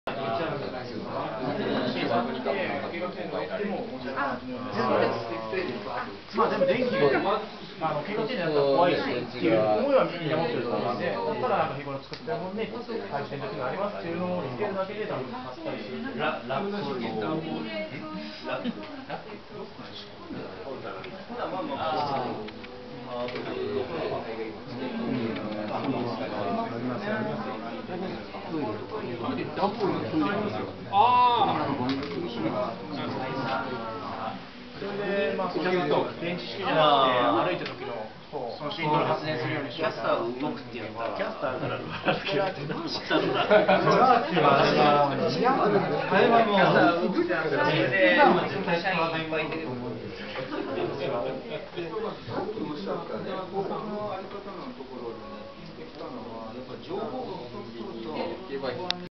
発電会議風景
発想トレーニングの場、及びクリエイティブ志向の人が集まる場として「発電会議」を発想しました。
そしてここから社会にインパクトを与える製品を産み出していきます。
説明
発電会議ファシリテーター
・有限会社東新製作所　石原　幸一 / tohshin seisakusho koichi ishihara
・株式会社enmono　三木　康司 / enmono kouji miki
・株式会社enmono　宇都宮　茂 / enmono shigeru utsunomiya

石原さんが主催している、東京都大田区の町工場2代目ネットワーク
おおたGroup Network

Field of thinking approaches and creative-minded people gather as "hatsudenkaigi" with the idea.